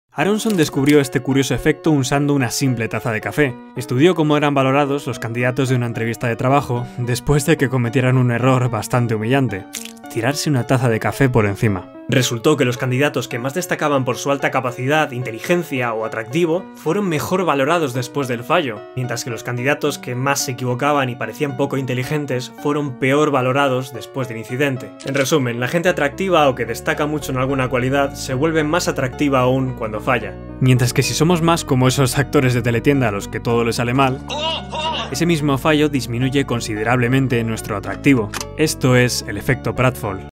PSICOLOGÍA SOCIAL ☕ Experimento: el EFECTO PRATFALL de Elliot Aronson
En este experimento, el psicolólogo social Elliot Aronson demostró que un error nos puede hacer mucho más atractivos